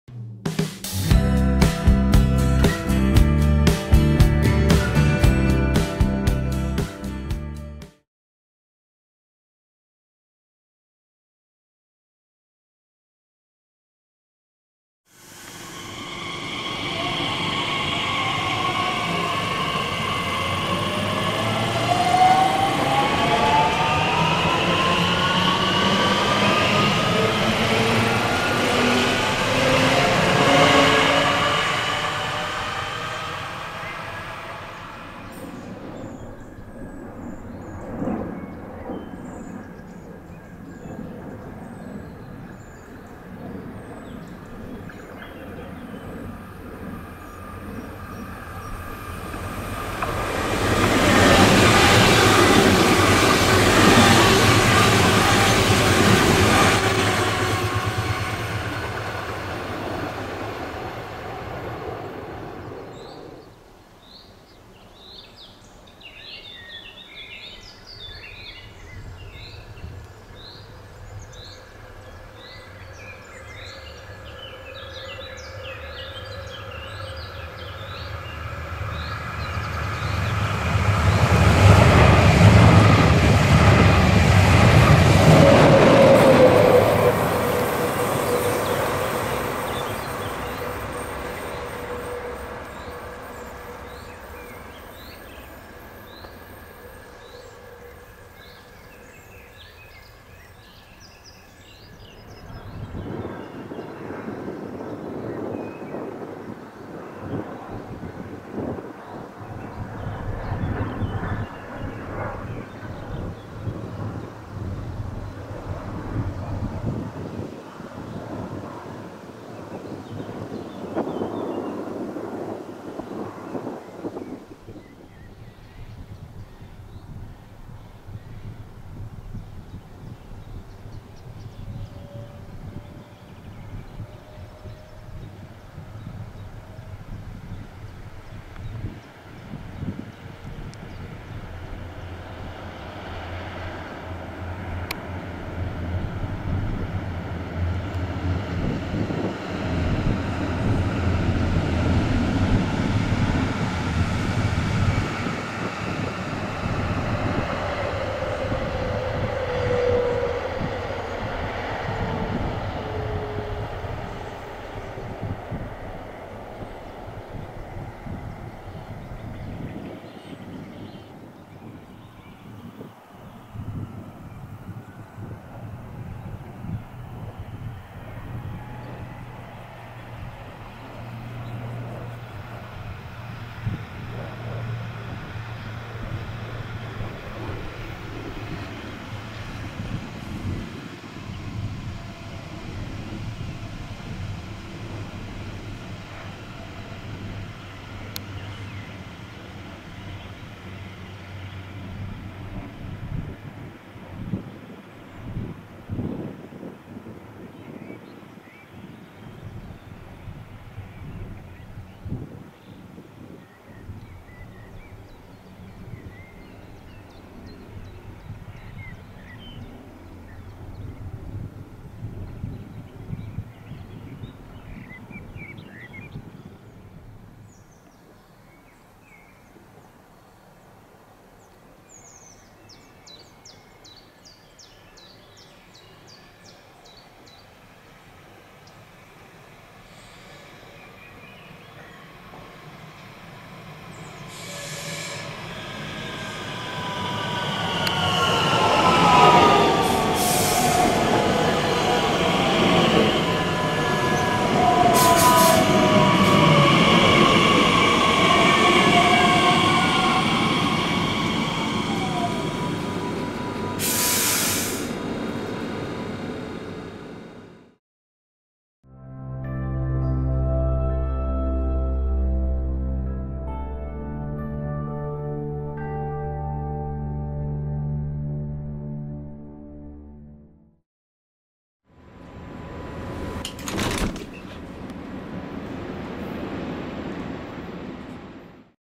Röthenbach Seespitze mit BR 143 samt x-Wagen und ET 442 der Nürnberger Ⓢ-Bahn
2020 waren die letzten x-Wagen Einsätze bei der Nürnberger S-Bahn, welche in diesem Video neben den ET 442 zu sehen sind. Die Hamster sind im deutschlandweiten Vergleich hier übrigens mit am längsten bereits seit Ende 2011 im Einsatz.

(Schlussszene: RB34 Ersatzzug)
English:
In 2020 there were the last services of x-Wagon trains in Nurembergs suburban train network. You can see them in this video besides DB class 442, those are already since 2011 in service, therefore the longest in Germany compared to other places where those trains are in usage.
Aufnahmedatum: Juni 2020

VERWENDUNG und Weiterverarbeitung dieses und aller anderen meiner Videos ist VERBOTEN. USAGE of this and any other of my Videos is FORBIDDEN.
© BergfelderVideos780